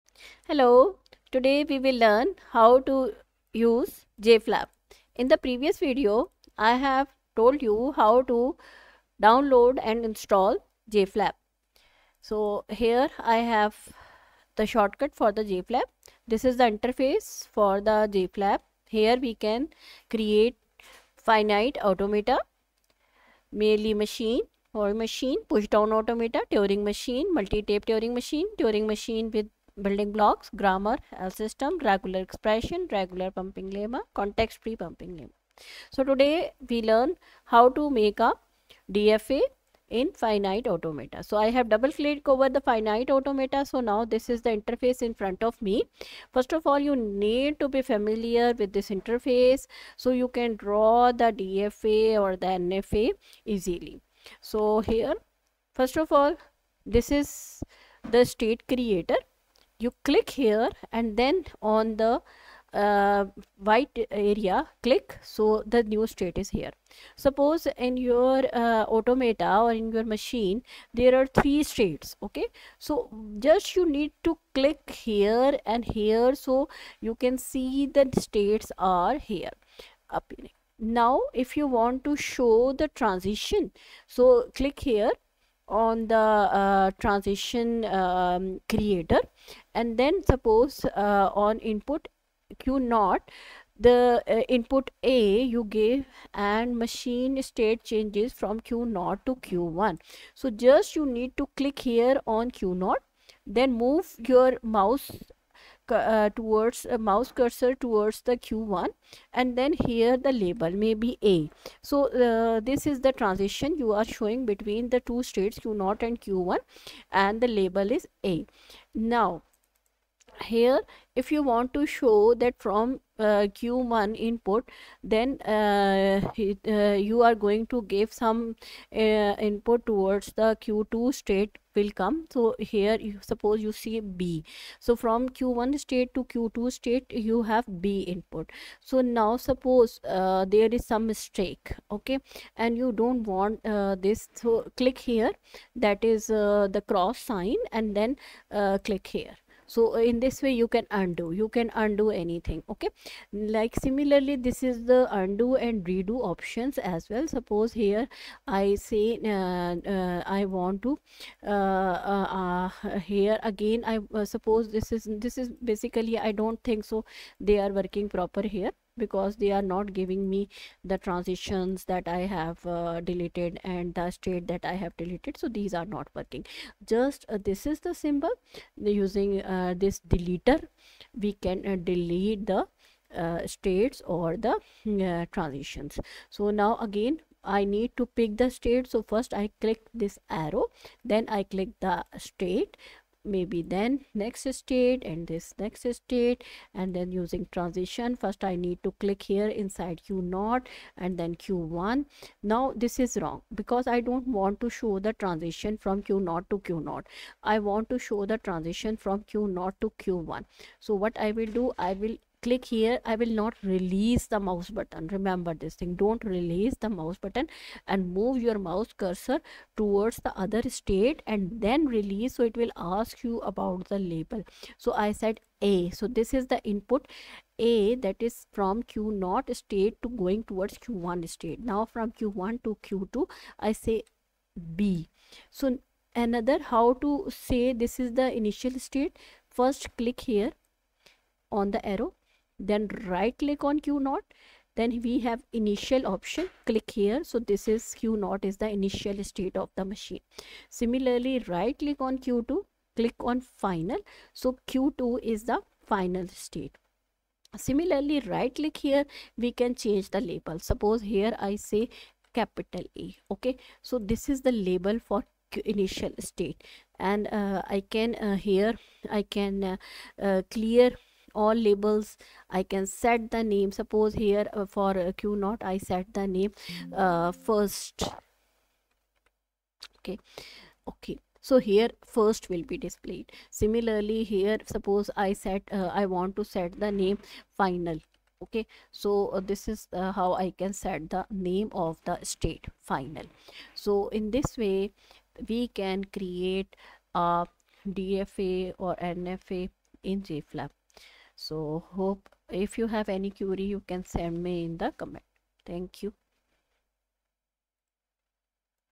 How to Use JFLAP?
Unlock the power of Finite Automata with this comprehensive tutorial on using JFLAP. Whether you're a beginner or looking to deepen your understanding, this video covers the essentials, from creating state diagrams to simulating automaton behavior. Follow along and gain the confidence to design and analyze finite automata efficiently. Dive into the world of JFLAP with this easy-to-follow tutorial and elevate your skills in automata theory!.How to change the label?how to change the name of the state?how to show the transitions on state diagram?how to delete a sate or transition?How to mark initial state and final state? all answers are available in this video.